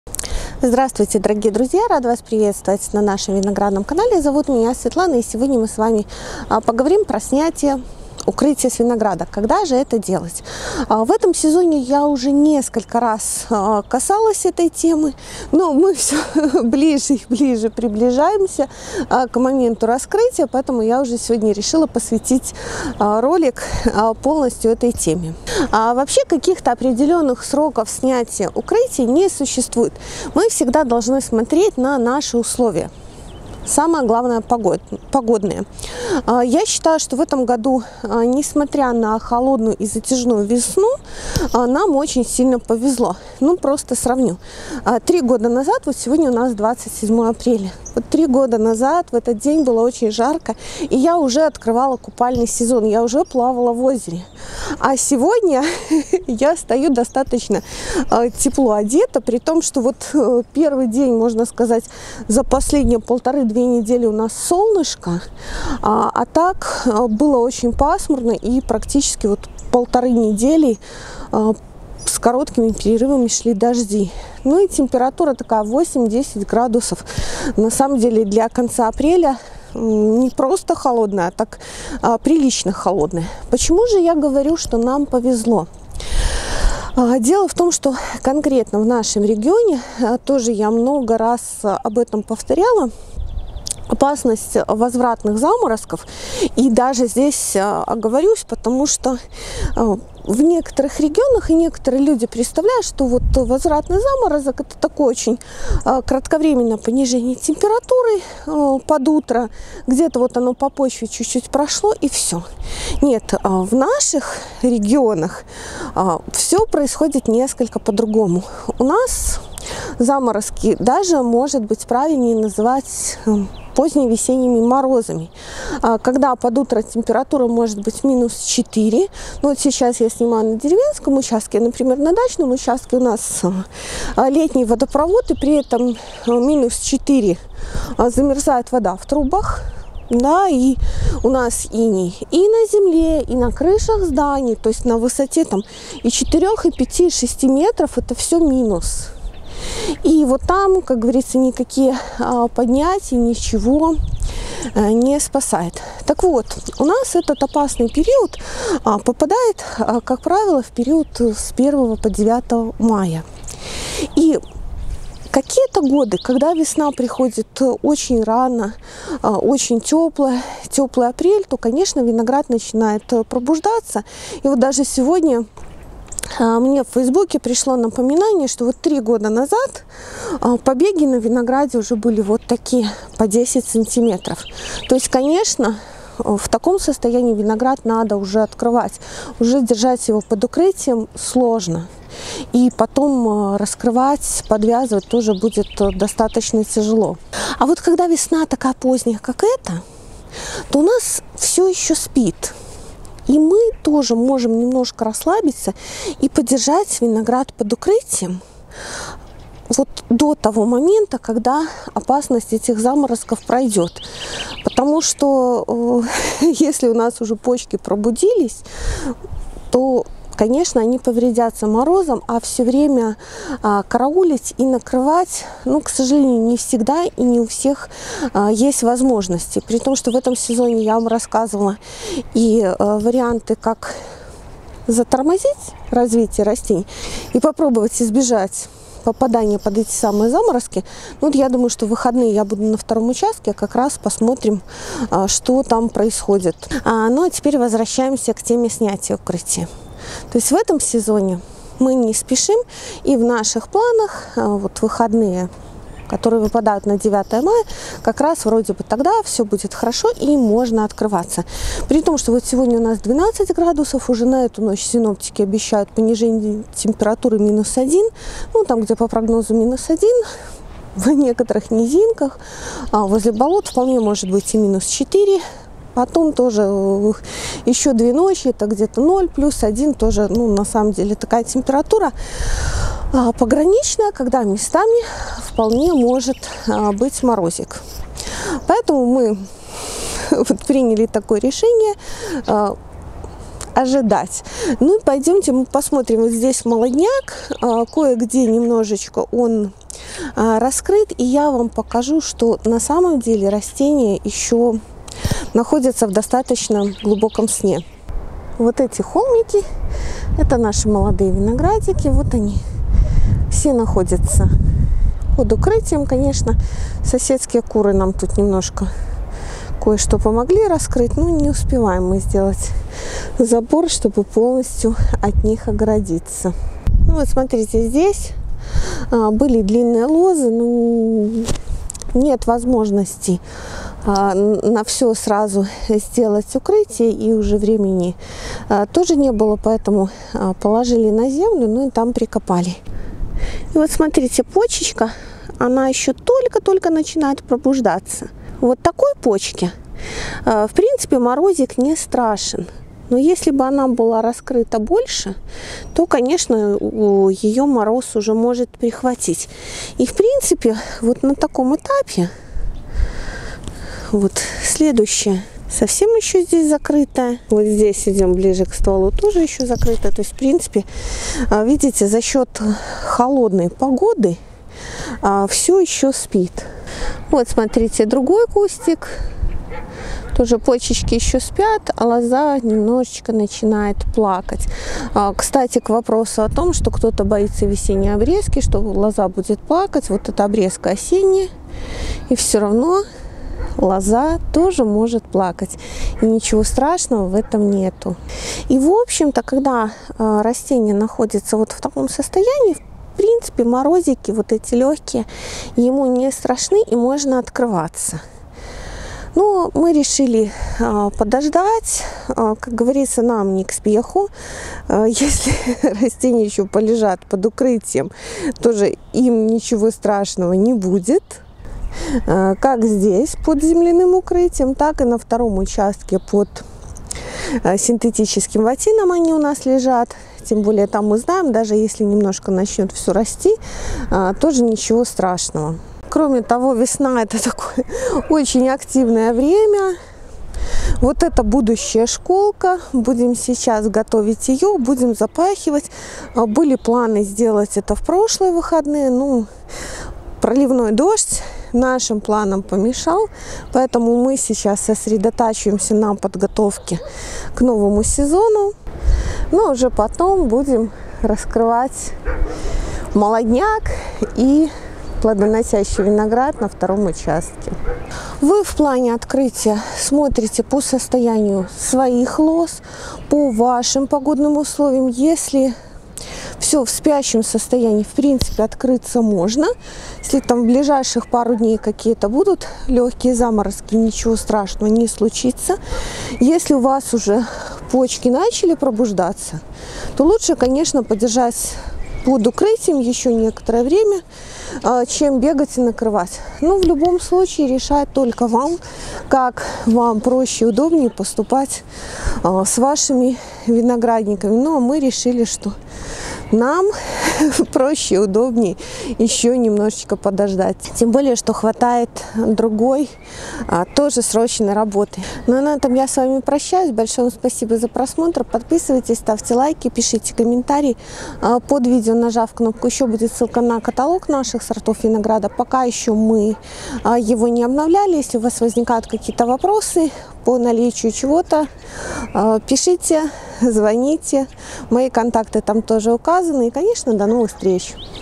Когда открывать виноград, сезон 2022. Мы с раскрытием не спешим
В какие сроки оптимально раскрывать виноград, на что обратить внимание и стоит ли спешить с раскрытием. Наши планы по раскрытию винограда